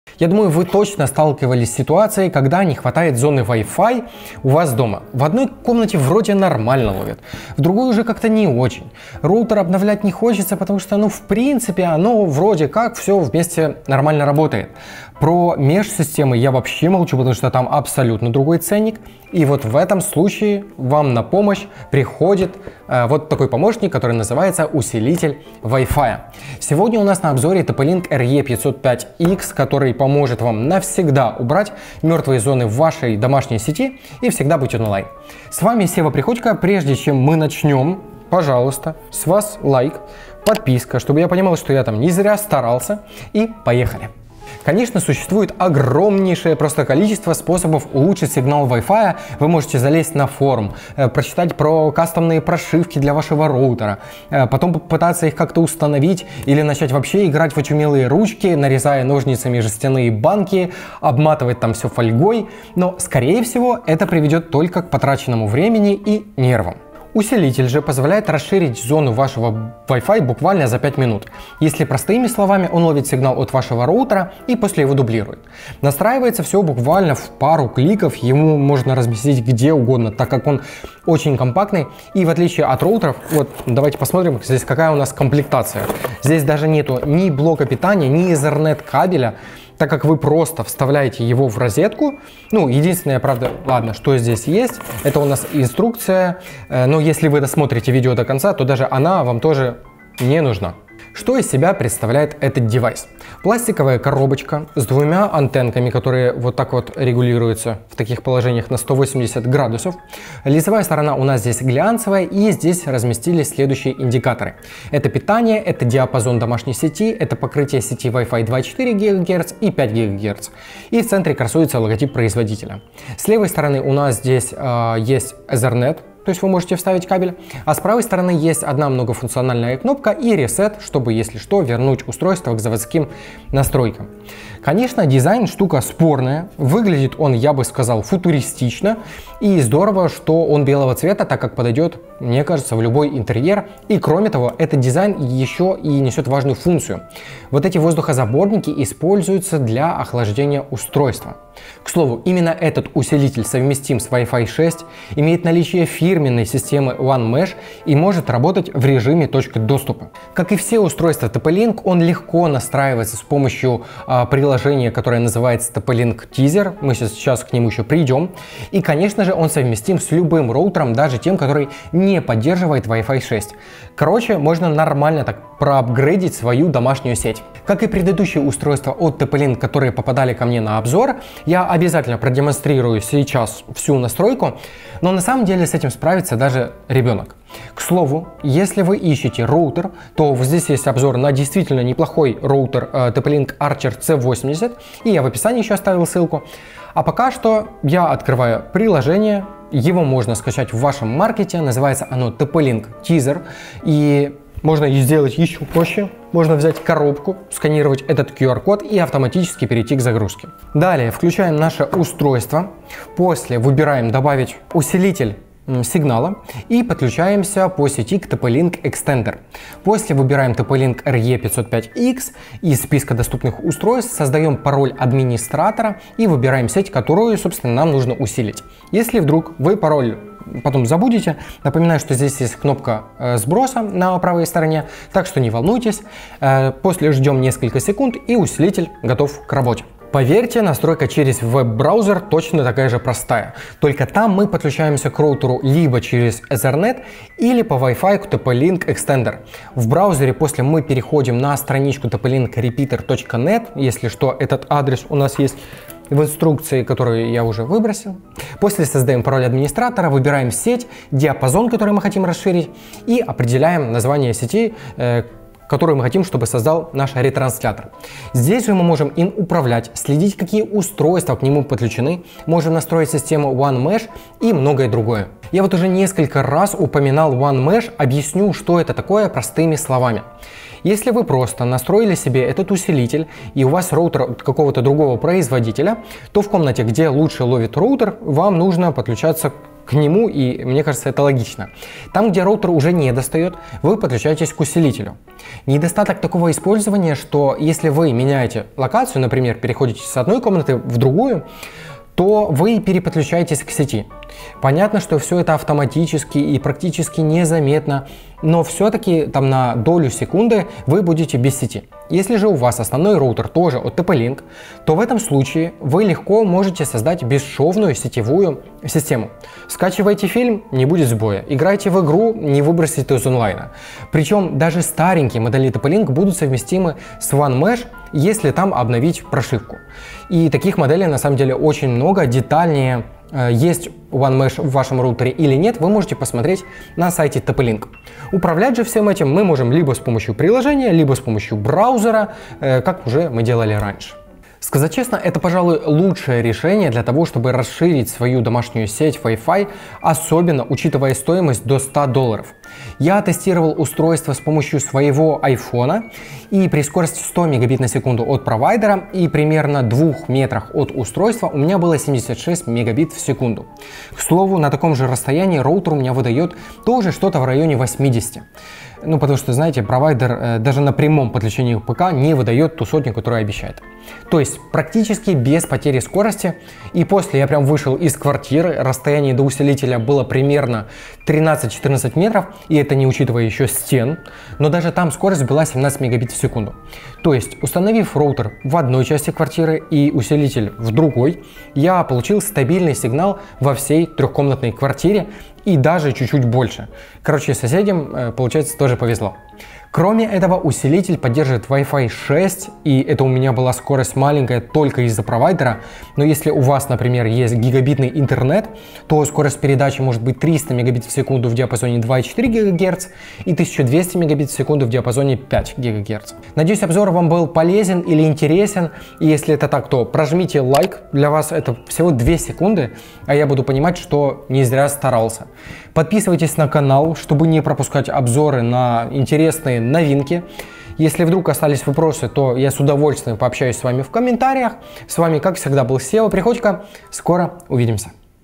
Усилитель Wi-Fi сигнала — TP-Link RE505X. Обзор, тесты и настройка.
Я думаю вы точно сталкивались с ситуацией, когда не хватает зоны Wi-Fi у вас дома. В одной комнате нормально ловит, в другой не очень. Роутер обновлять не хочется, потому что вроде бы нормально работает и именно в таком случае на помощь приходит усилитель Wi-Fi сигнала. Сегодня у меня на обзоре TP-Link RE505X, который поможет вам навсегда убрать «мертвые» зоны в вашей домашней сети и всегда быть онлайн.

00:00 Вступление
00:44 Какие бывают способы усилить Wi-Fi?
01:25 Комплектация TP-Link RE505X
02:27 Как устроен усилитель Wi-Fi?
03:15 Настройка усилителя с помощью мобильного приложения
04:00 Настройка TP-Link RE505X с помощью браузера
04:35 Что такое OneMesh?
05:39 Как управлять усилителем?
05:53 Тесты устройства и скорости
06:37 Итоги обзора

Конечно существует огромное количество способов улучшить сигнал Wi-Fi, но скорее всего это приведет только к потраченному времени и нервам. Усилитель же позволяет расширить зону Wi-Fi буквально за 5 минут. Если простыми словами, он ловит сигнал от вашего роутера и дублирует его. Настраивается все буквально в пару кликов, его можно разместить где угодно, т.к. он очень компактный.

Именно этот усилитель совместим с Wi-Fi 6 имеет наличие фирменной системы OneMesh и может работать в режиме точки доступа. Как и все устройства TP-Link он легко настраивается и работает с помощью мобильного приложения Tether и конечно же он совместим с любым роутером, даже тем который не поддерживает Wi-Fi 6.

Сказать честно, это пожалуй самое лучшее решение для того чтобы расширить свою домашнюю сеть Wi-FI, особенно учитывая стоимость до 100$. Я тестировал устройство с помощью своего смартфона и при скорости 100 мб/с от провайдера и примерно в двух метрах от устройства у меня было 76 мегабит в секунду. К слову на таком же расстоянии роутер у меня выдает тоже что-то в районе 80. Потому что провайдер даже на прямом подключении к ПК не выдает сотню. Т.е. практически без потери скорости.

Установив роутер в одной части квартиры и усилитель в другой я получил стабильный сигнал во всей трехкомнатной квартире и даже чуть-чуть больше. Кроме этого, усилитель поддерживает Wi-Fi 6 и это у меня скорость была маленькая из-за провайдера, но если у вас гигабитный интернет, то скорость передачи может быть 300 Мб/с в диапазоне 2,4 ГГц и 1200 Мб/с в диапазоне 5 ГГц.